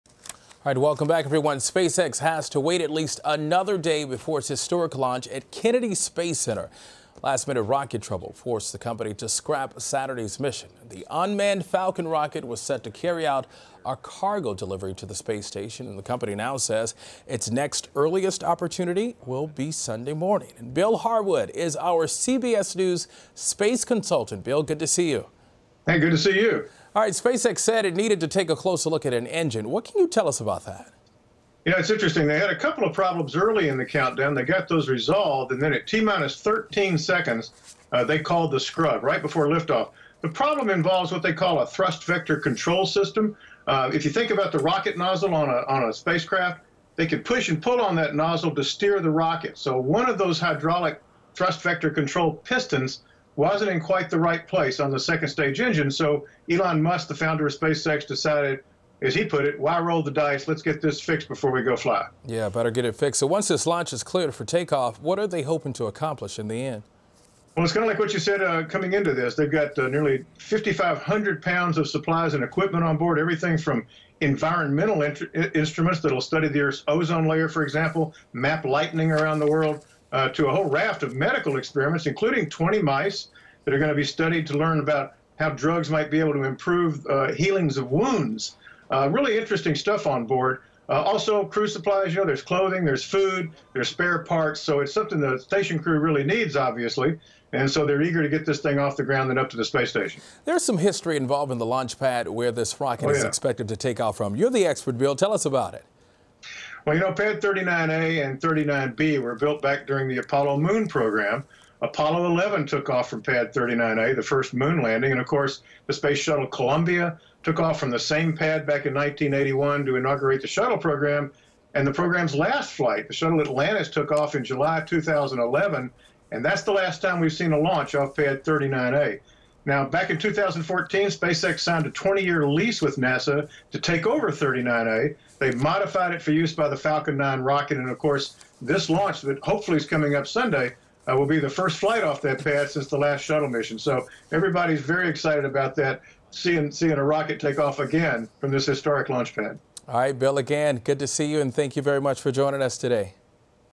SpaceX postpones rocket launch seconds before takeoff
Just seconds before blastoff, SpaceX canceled a launch of its Falcon 9 rocket. CBS News space consultant Bill Harwood breaks down why the launch fell through.